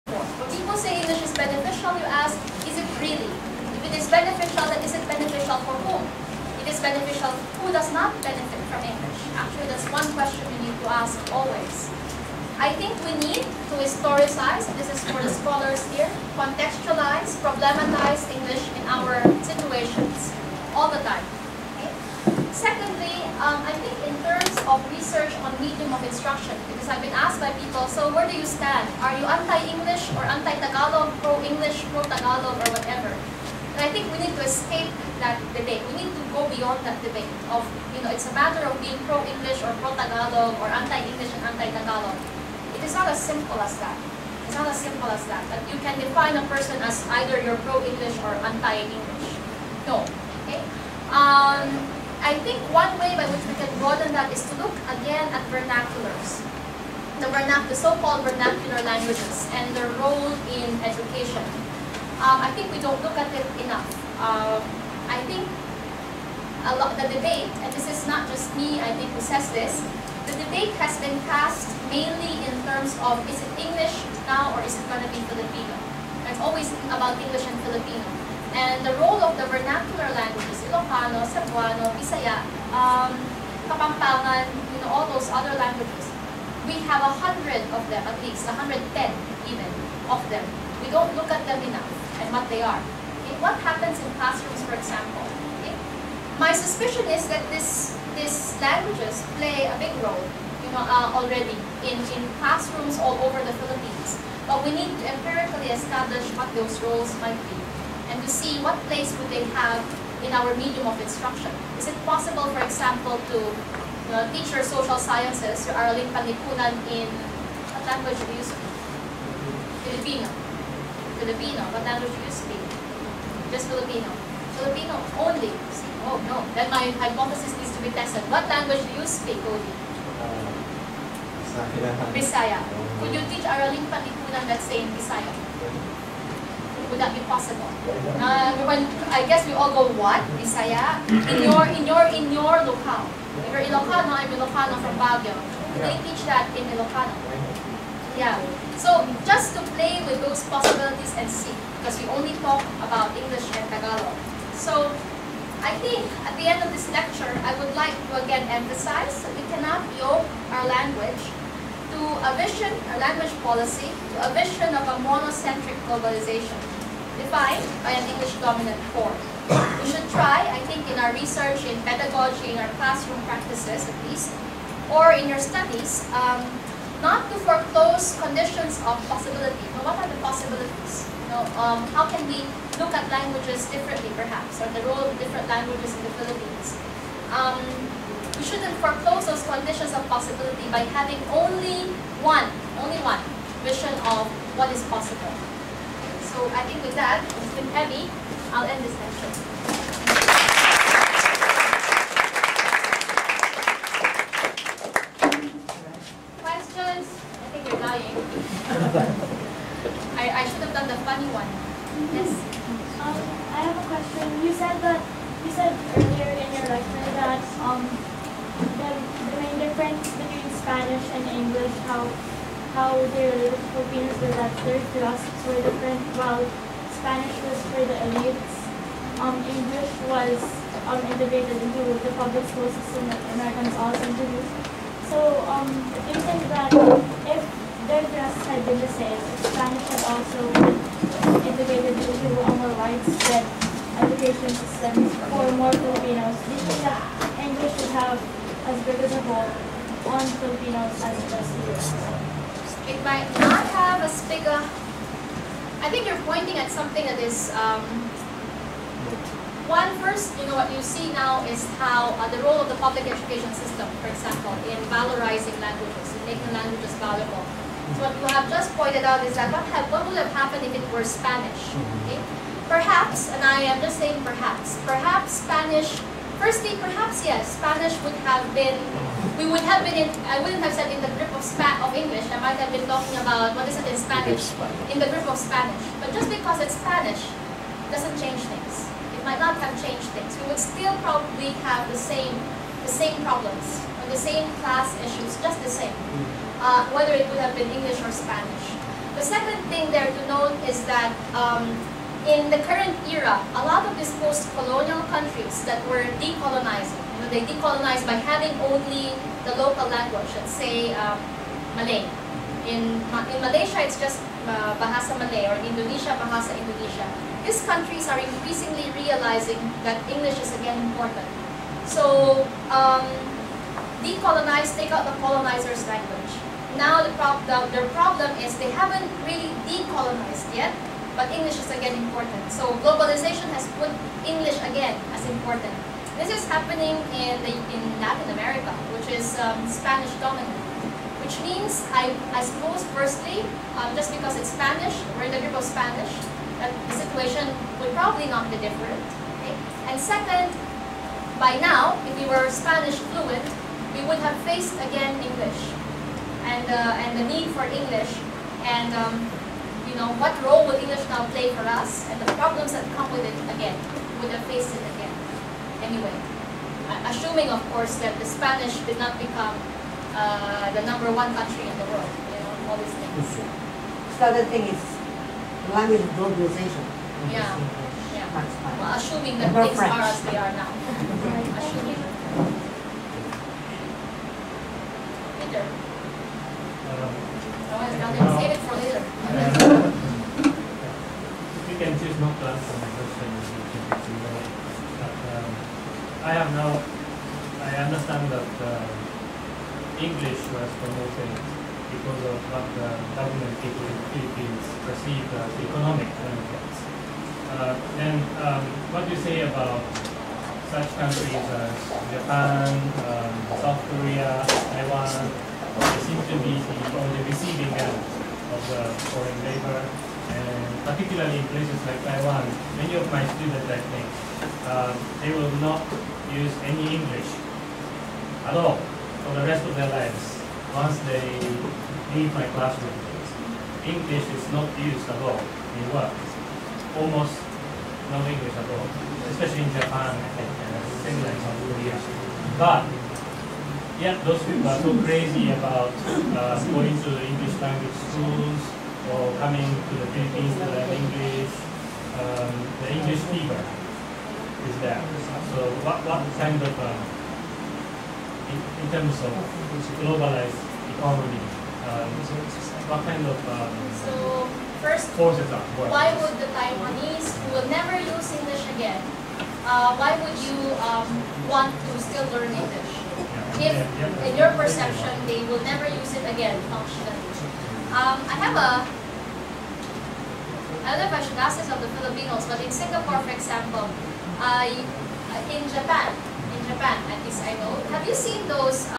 KK Lecture: Beatriz Lorente (May 2008) Part 1
KK Lecture Series "In the Grip of English: The Impact of Migration on Language Policy in the Philippines." Date: 12 May 2008. Venue: Ateneo de Manila University.